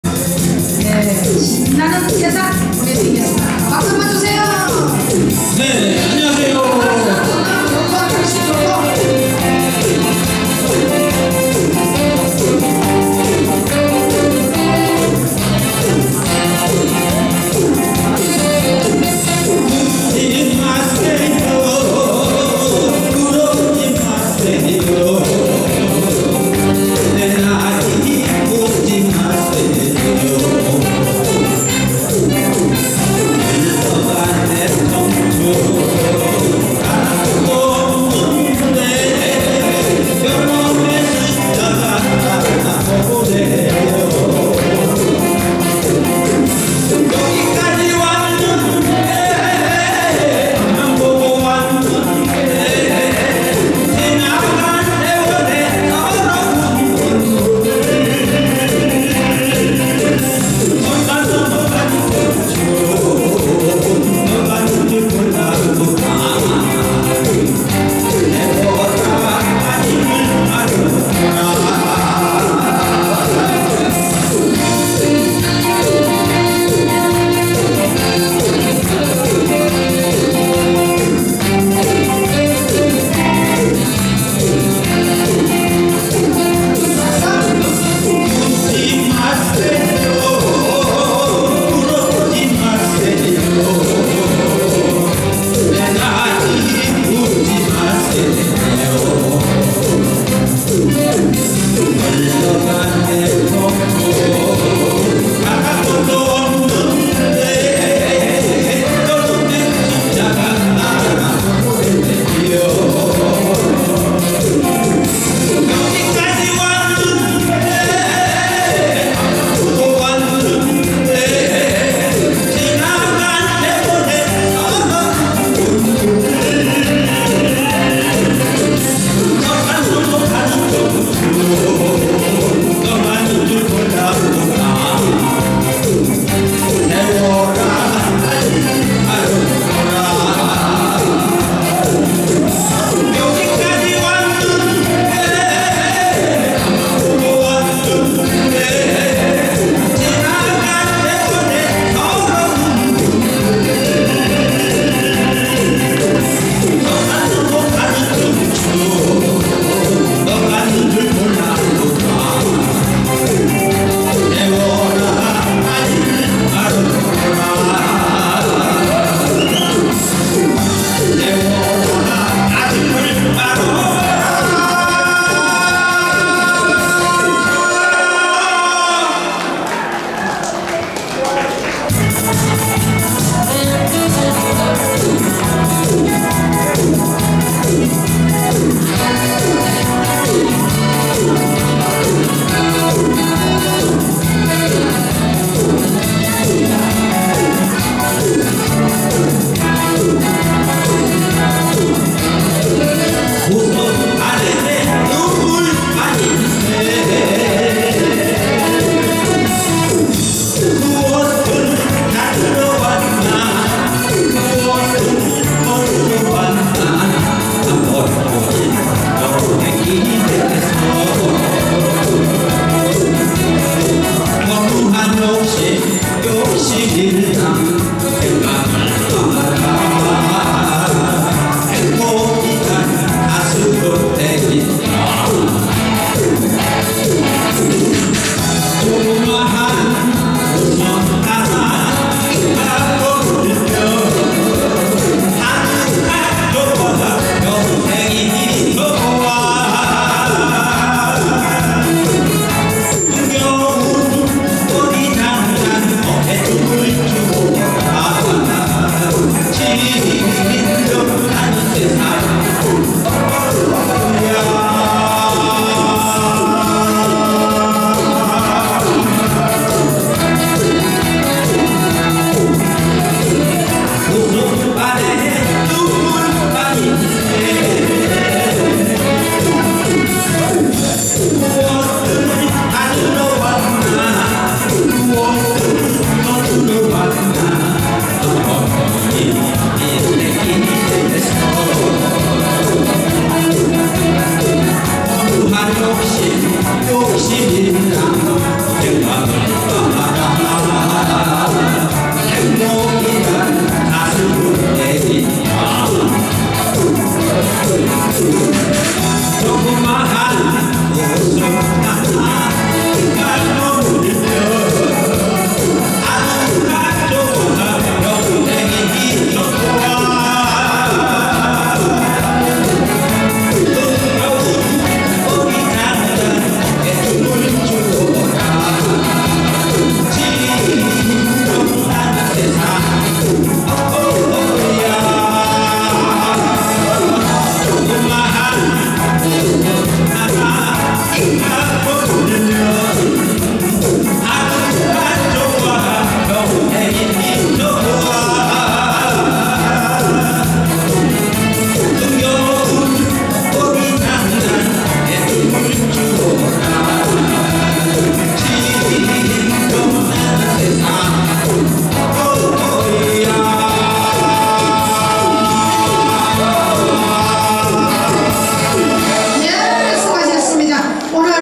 새마음(장학회)예술단 정기공연(가수 이선구 ) 장소청계천 마장교 일시2016.4.5
새마음(장학회)예술단 정기공연(가수 이선구 )
장소청계천 마장교
일시2016.4.5